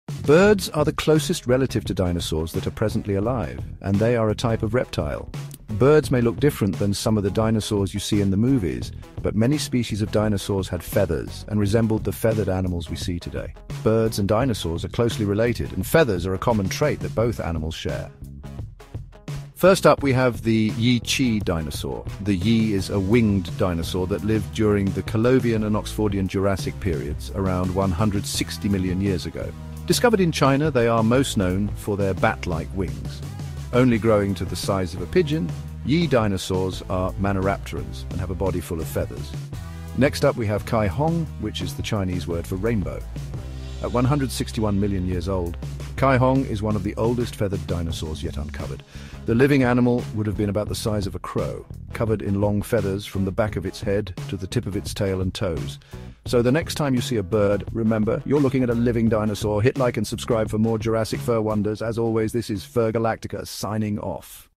Qi Yi and Caihong two unique Dinosaur 🦕 Discoveries
Discover two fascinating feathered dinosaurs—Qi Yi and Caihong! Learn how these prehistoric creatures used their fur-like feathers for display, warmth, or flight evolution. Dive into groundbreaking paleontology research, vibrant fossil discoveries, and what makes these dinosaurs unique. Perfect for dinosaur enthusiasts, fossil hunters, and fans of prehistoric mysteries!